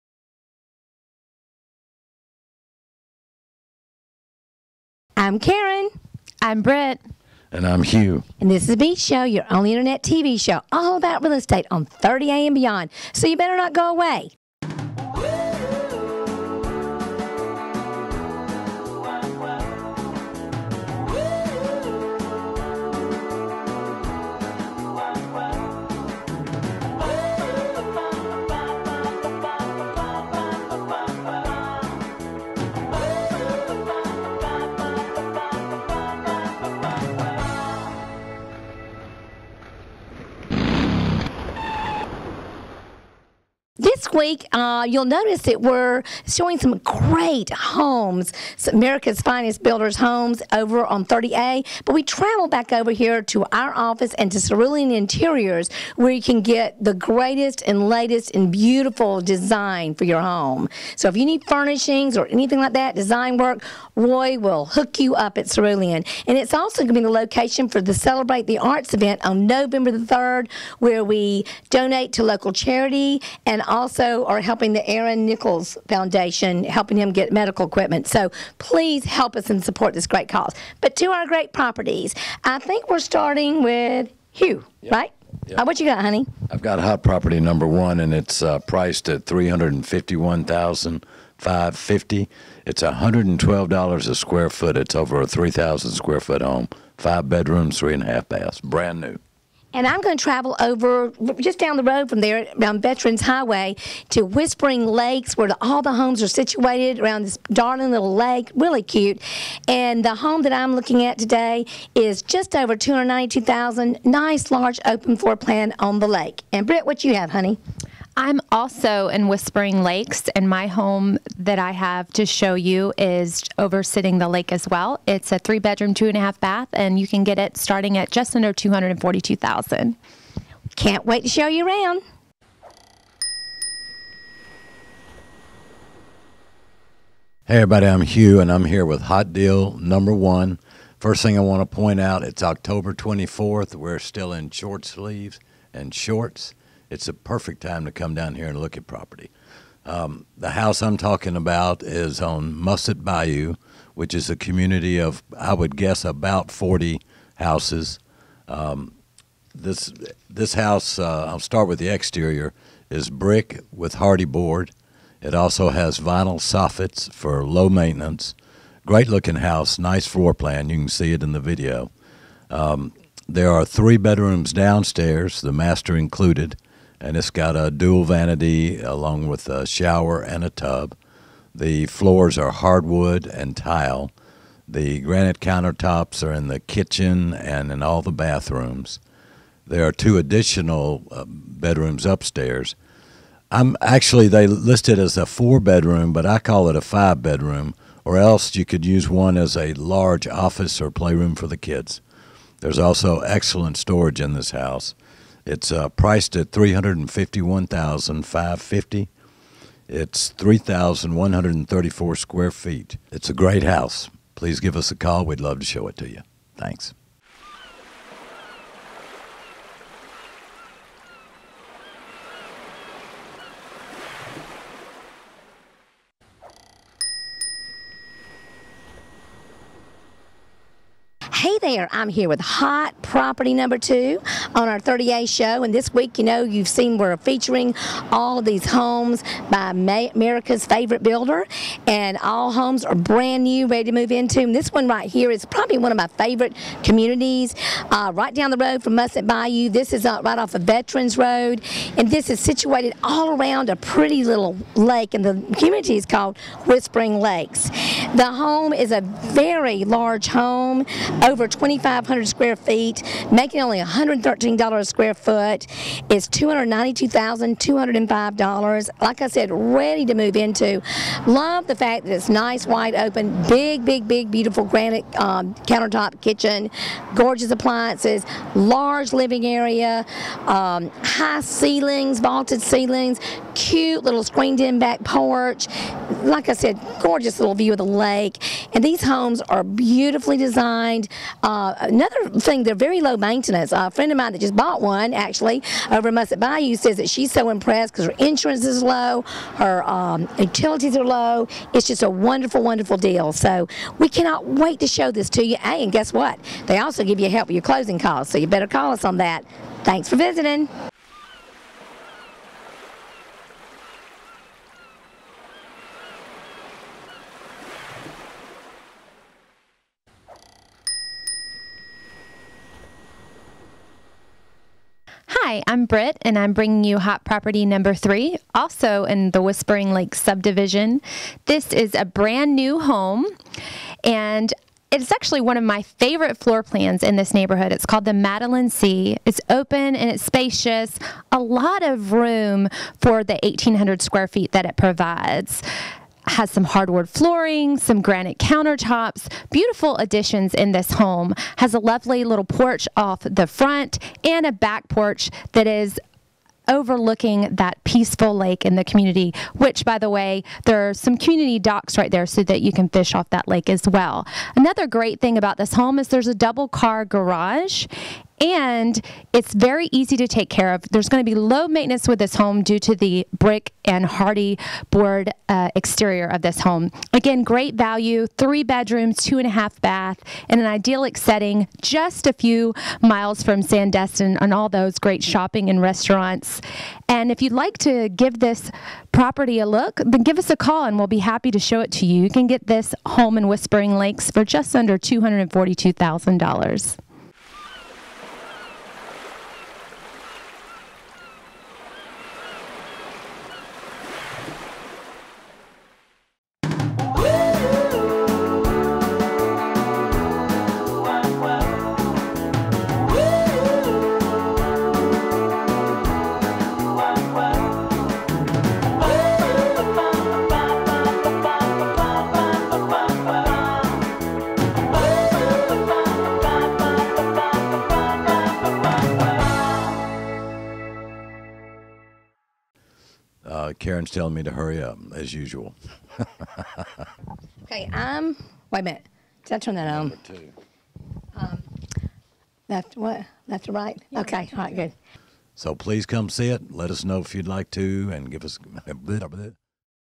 The Beach Show 30A - #56 - 30A Real Estate - Florida
Hot property #1 is a brand new home in Musset Bayou for $351,550

Over 3,134 square feet with side entry double garage

top of line cabinetry with gorgeous granite

4 bedrooms with a 5th large room and 3.5 baths

Large living space with open floor plan and Hardwood flooring.

Hot property #2 is a lake home for $292,205

Over 2,500 square feet with 4 bedrooms and 2.5 baths

ONLY 113 per square foot!

situated on a peaceful little lake with a screened in back porch

Large open living area with wide open kitchen

Hot property #3 is another Lake front home in Whispering Lake for only $241,935

3 bedroom 2 baths

Lake front! Tranquil, peaceful, quiet and restful.

This is the Madeline C plan that is very popular and has an open feeling with lots of light pouring in.

Low maintenance Hardi board siding with gorgeous granite interior and finishes